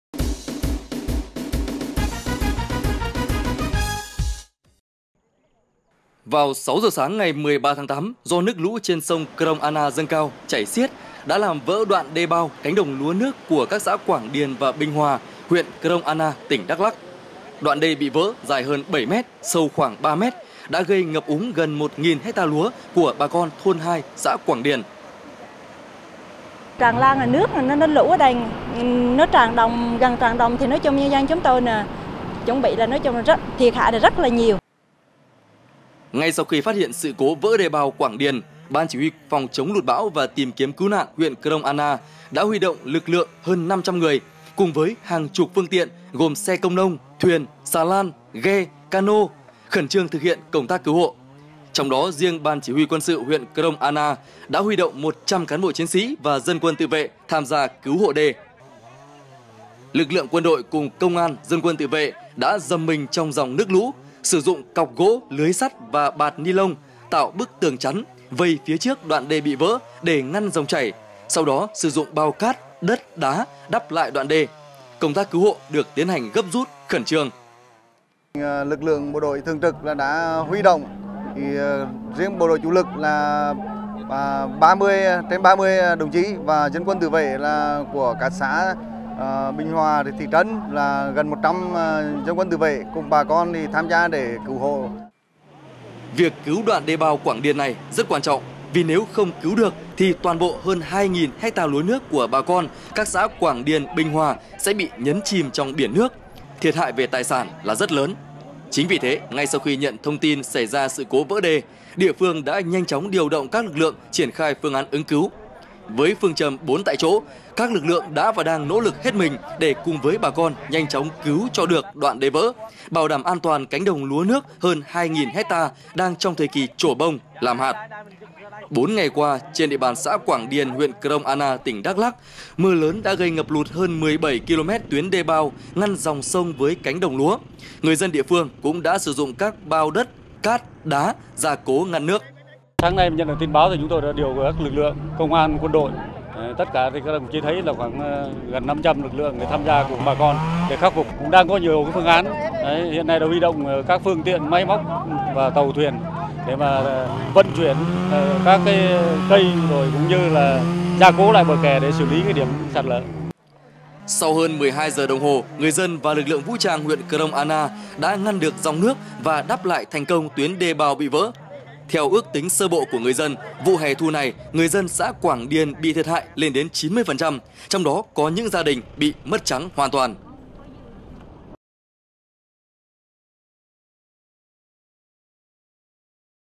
Quân, dân gồng mình hộ đê, cứu lúa đoạn qua xã Quảng Điền, huyện Krông Ana, Đắk Lắk
Quân, dân gồng mình hộ đê, cứu lúa
QĐND Online - Do ảnh hưởng của mưa lũ, khoảng 5 giờ sáng 13-8, một đoạn đê bao Quảng Điền (đoạn qua xã Quảng Điền, huyện Krông Ana, Đắk Lắk) đã bị vỡ hơn 10 mét.